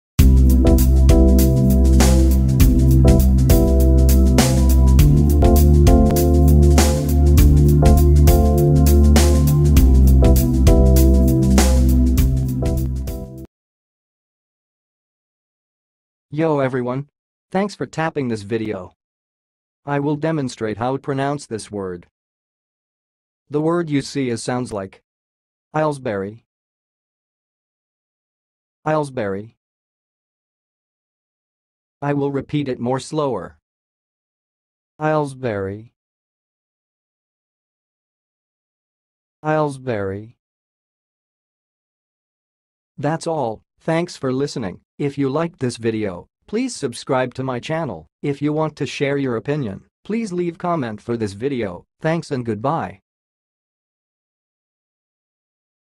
AILESBURY - How to pronounce it?
AILESBURY - Say Ailesbury like an expert: Pronunciation guide in 2023

Ailesbury is a historical term that refers to the estate or lands associated with the Ailesbury family.
It could also specifically refer to a place named Ailesbury, which often indicates a connection to the Ailesbury family.
The term is mainly used in the context of British history and genealogy.

Pronunciation:
To pronounce Ailesbury, you can say it as ey-uhlz-buh-ree or eylz-buh-ree.
Another variant is eylz-ber-ee.

Test yourself by speaking the following examples:
1. The Ailesbury family once owned vast lands in the English countryside
2. The Ailesbury estate is known for its beautiful gardens
3. Ailesbury history is closely linked to British aristocracy
4. The village of Ailesbury is situated in a picturesque location